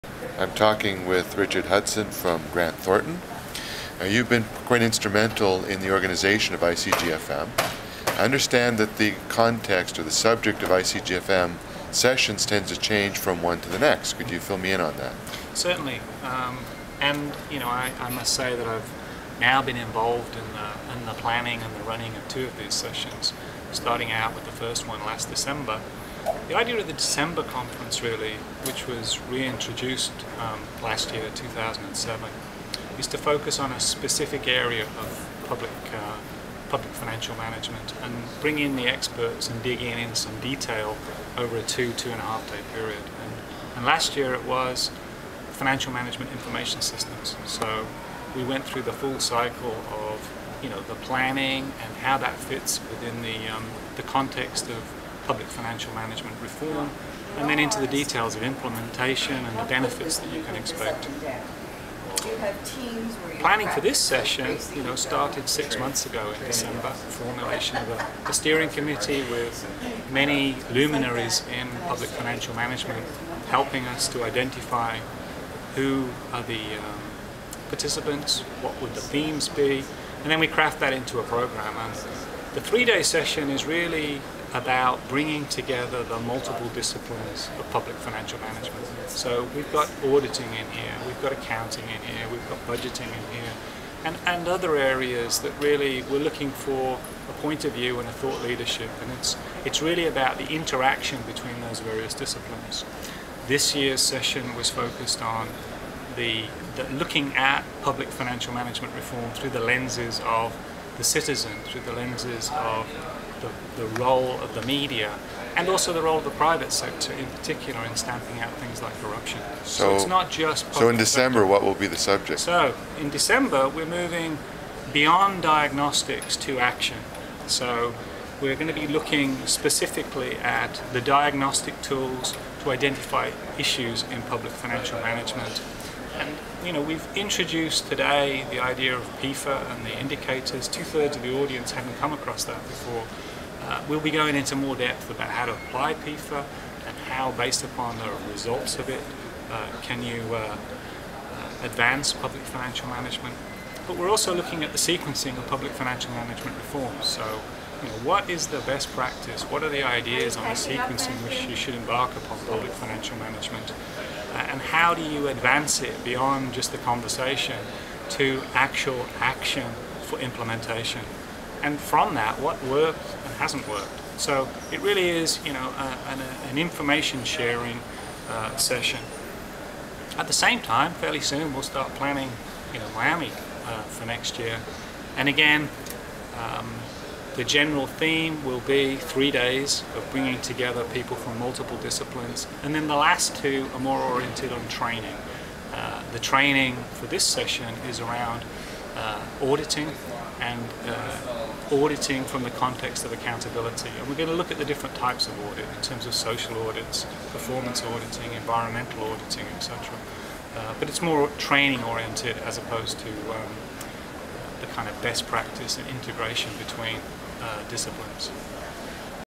Richard Hudson describes the focus of ICGFM conferences
Richard Hudson talks about how ICGFM conferences are organized and the basic structure for the December conference